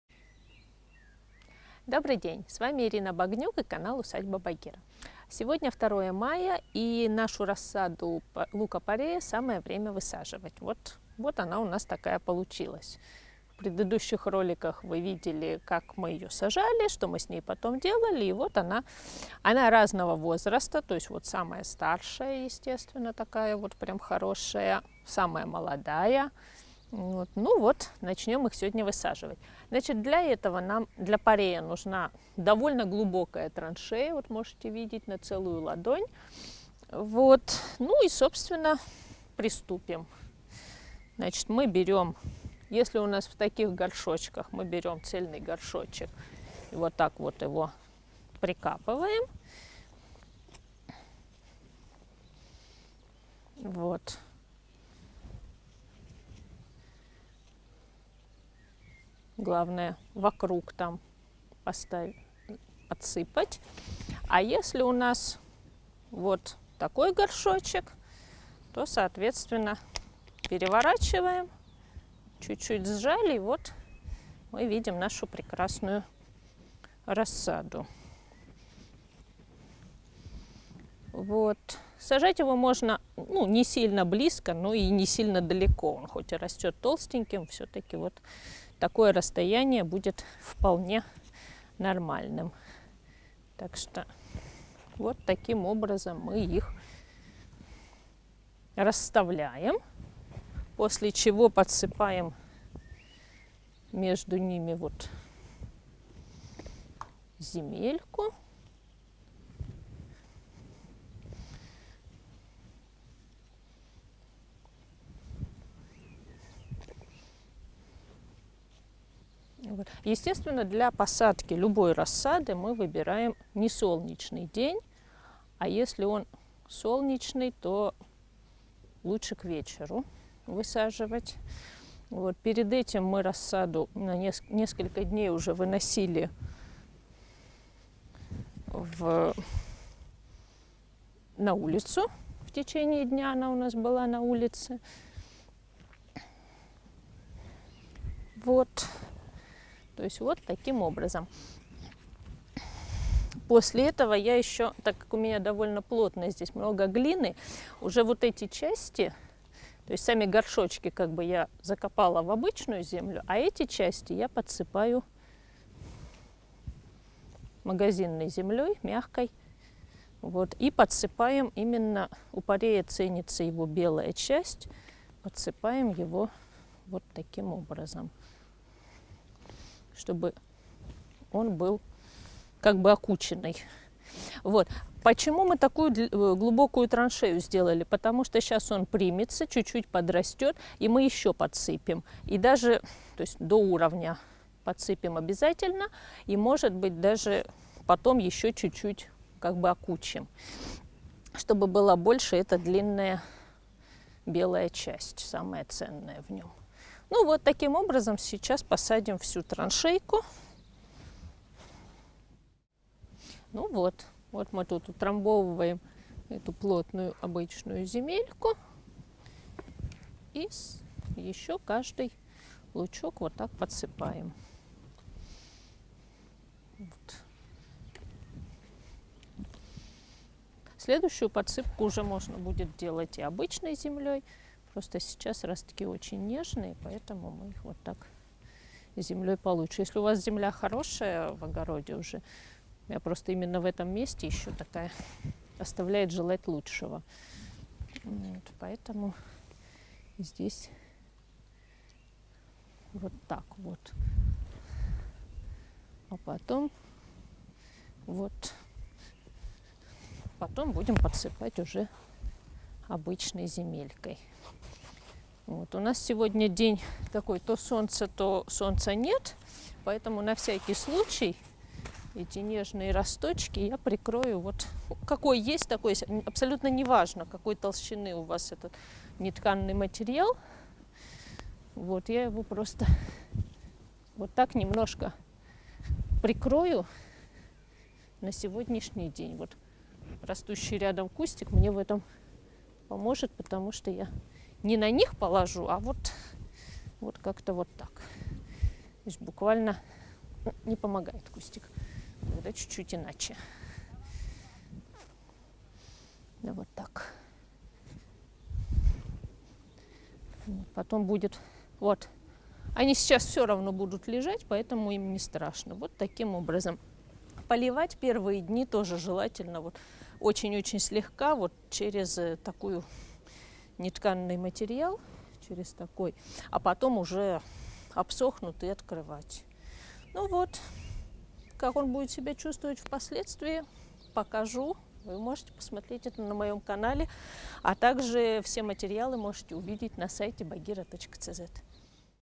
Посадка лука-порея в грунт
Рассада лука-порея подросла и готова к высадке в открытый грунт. Так как это довольно холодостойкая культура, высаживать его можно уже в начале мая, чем мы сегодня и займемся.

Канал Telegram - новости, общение, оповещения
Канал Telegram - личное общение, заказ консультаций